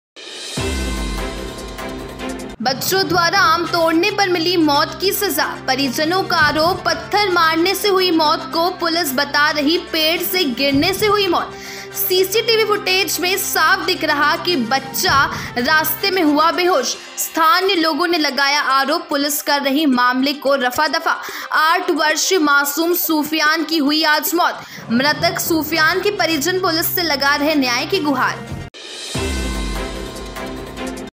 भोपाल - बच्चो द्वारा आम तोड़ने पर मिली मौत की सज़ा
बच्चो द्वारा आम तोड़ने पर मिली मौत की सज़ा।

परिजनों का आरोप पत्थर मारने से हुई मौत को पुलिस बता रही पेड़ से गिरने से हुई मौत।

सीसीटीवी फुटेज में साफ दिख रहा कि बच्चा रास्ते में हुआ बेहोश।

स्थानीय लोगों ने लगाया आरोप पुलिस कर रही मामले को रफा-दफा,

8 वर्षीय मासूम सुफियान की हुई आज मौत।

मृतक सुफियान के परिजन पुलिस से लगा रहे न्याय की गुहार।